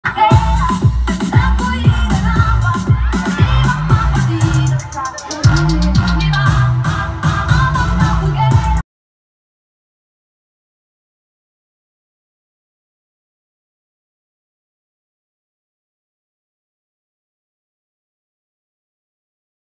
Sexy Girl FALLS in high heels during "Best in Swimsuit" Competition of a Filipino Beauty Pageant
During the "Best in Swimsuit" competition of a beauty pageant held in the Philippines in September 2022, the candidate n°6 tripped on her very high heels and completely fell down, losing her delicate sheer cape in the process!
Slow motion from 0:09.